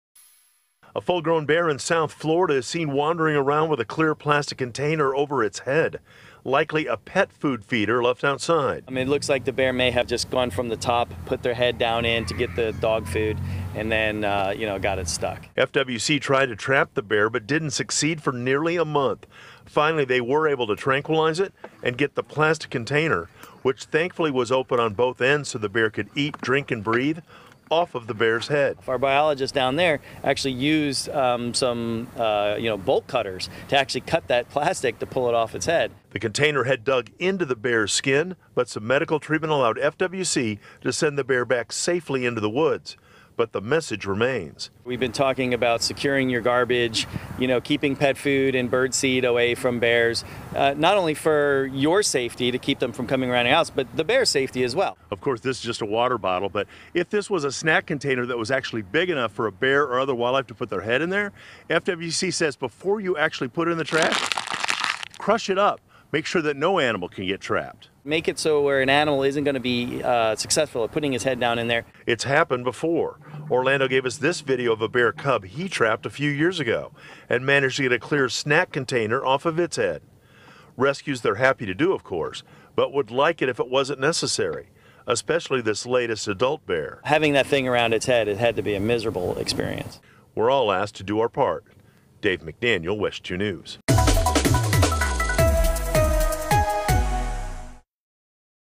Bear finally freed after month with head stuck in plastic container
This bear was wandering the streets of Florida, US, for weeks on end with a plastic container stuck on its head, until local authorities came to the rescue.